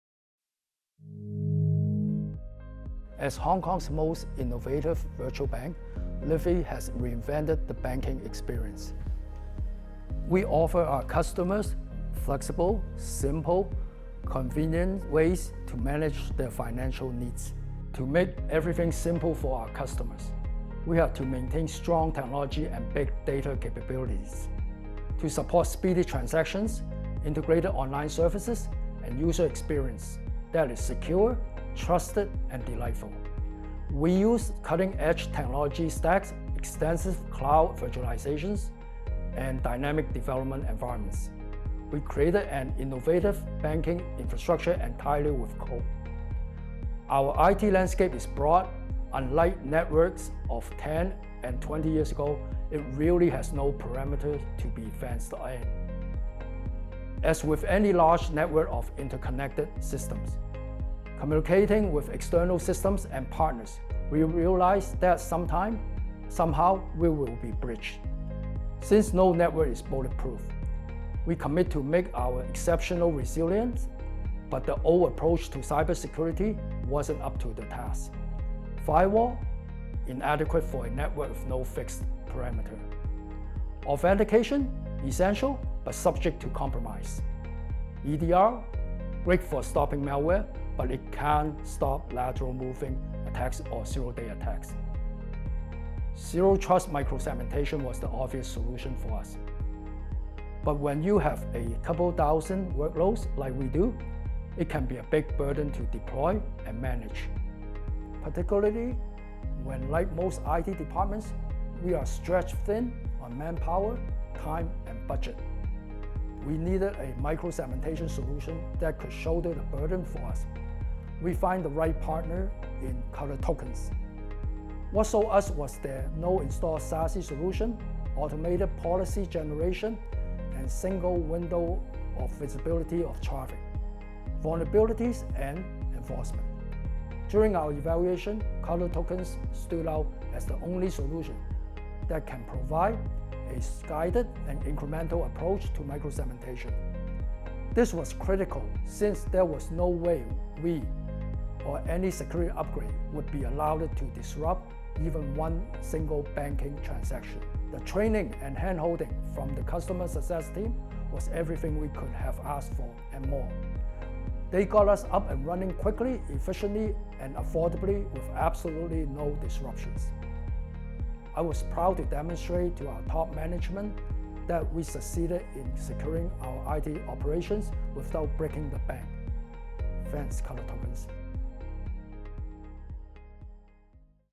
Securing Hong Kong's Most Innovative Virtual Bank: Livi Bank's Microsegmentation Journey
Anthony Fung, Chief Information Security Officer at Livi Bank, Hong Kong's virtual banking leader, shares their incredible journey of adopting microsegmentation to secure their complex IT infrastructure. Learn how they overcame challenges, achieved zero trust security, and stopped hackers in their tracks, all without disrupting a single transaction with ColorTokens.